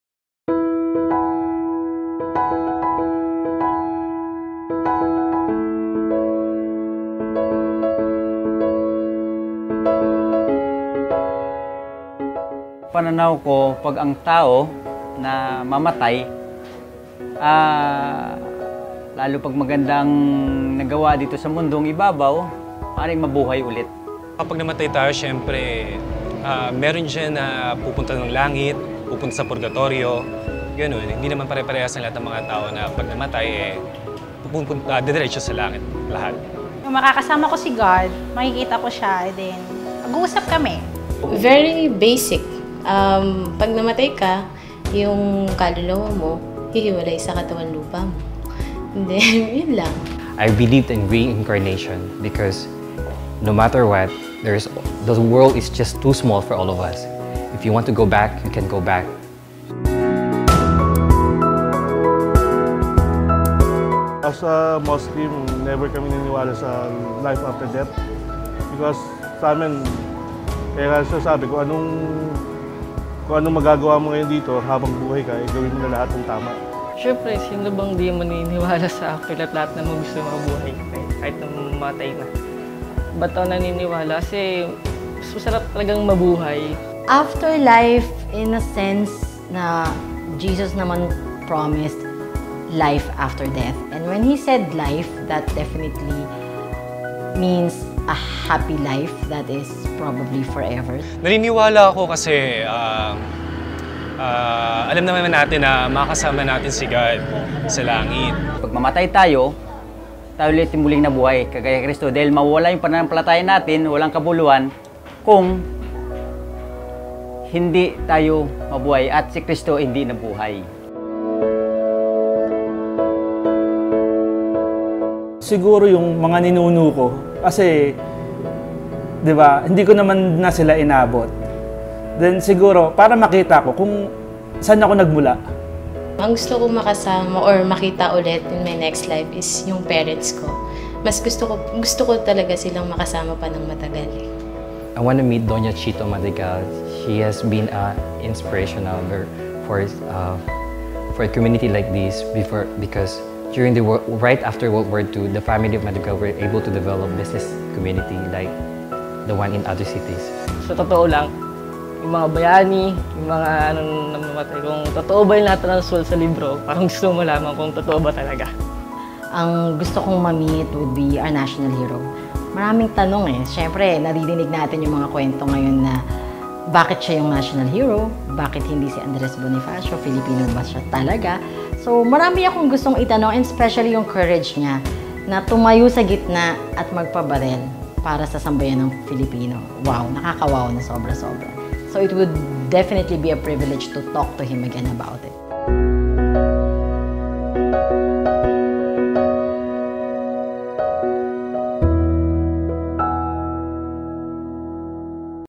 Beyond Death - Is there an afterlife?
Is heaven real?  Where do we go when we die?  People share their thoughts on life after death.